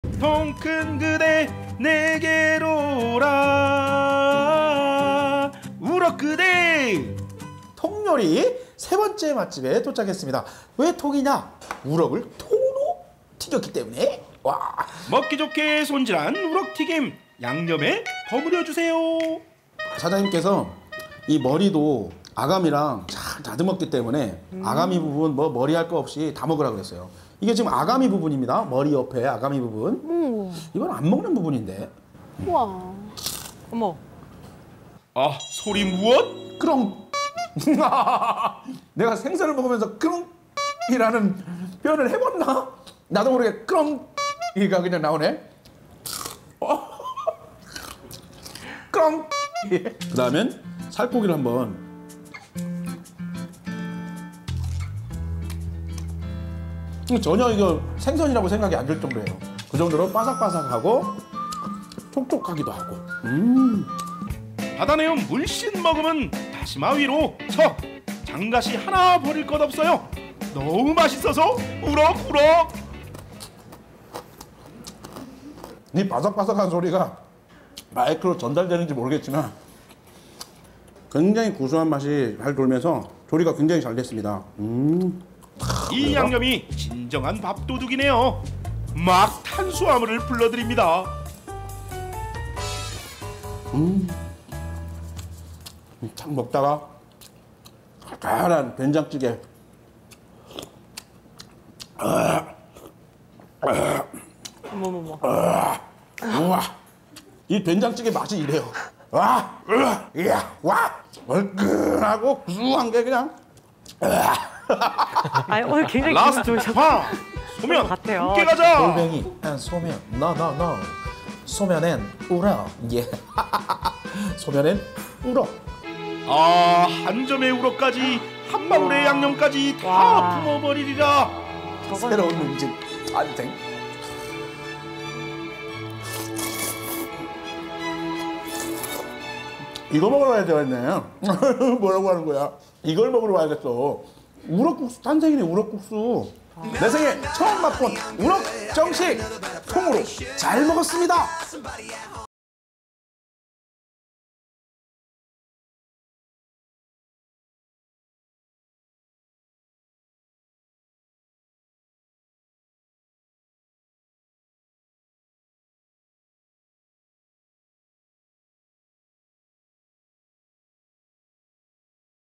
우럭도 튀겨야 맛있다! 제주 우럭정식 원조 '월정어촌계식당'
월정어촌계식당

영업시간 : 매일 08:30-21:00 연중무휴
주소 : 제주 제주시 구좌읍 월정서로 169